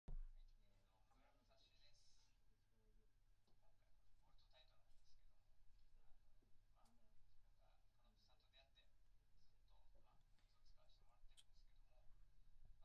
CANOPUS Speed Master Bearing SS-2000
Here are comparison video between speed star bearing and original installed bearing.
Please check out the difference of each pedal action.

Pedal : Pearl P-2000